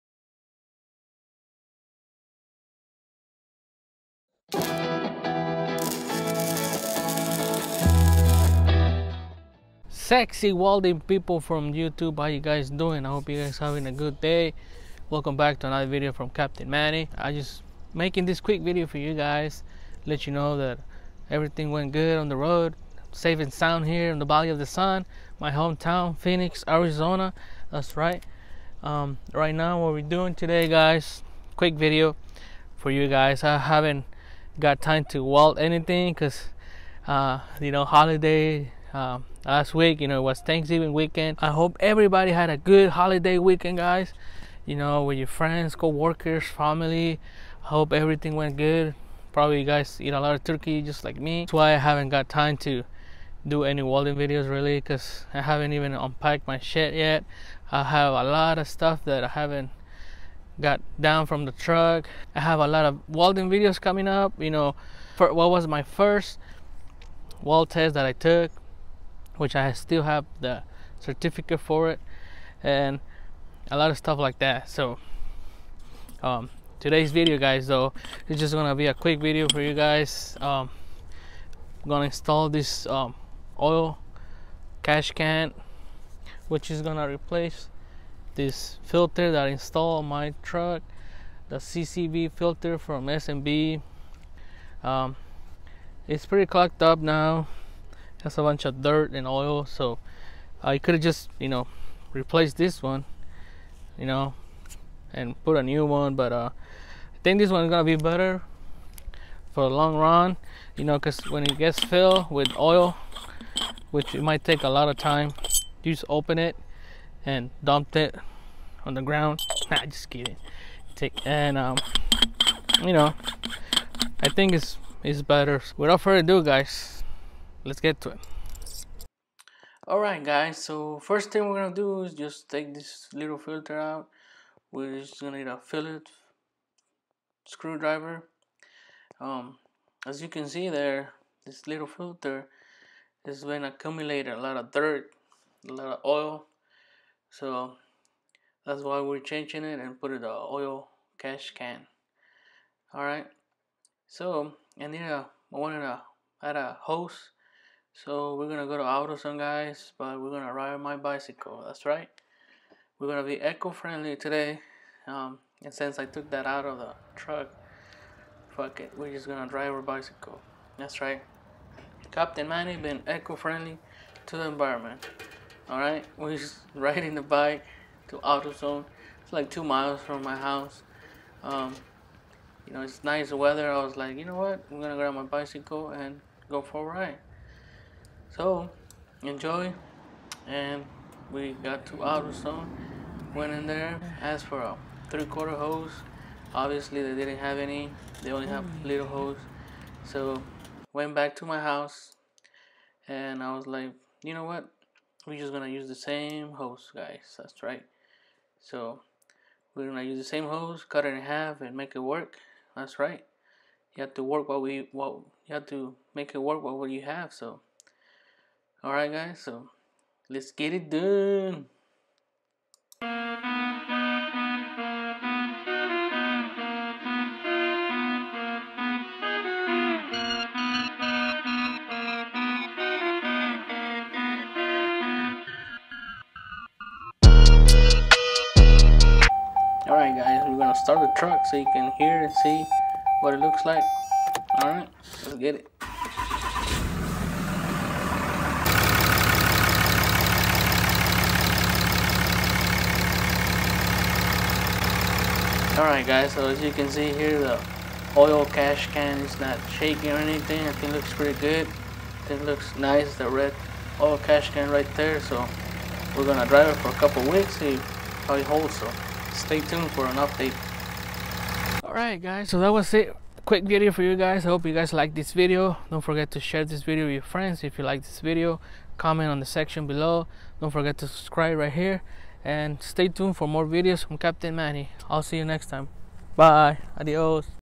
If you have a 6.7 , You need this to Prevent Damage to your Engine-oil Catch Can
hello Sexy welding people from youtube!!, if you have a 6.7 Cummings engine this mod is a must have!!
oil catch can ,you need this to prevent Damage to your engine.prevent carbon build up .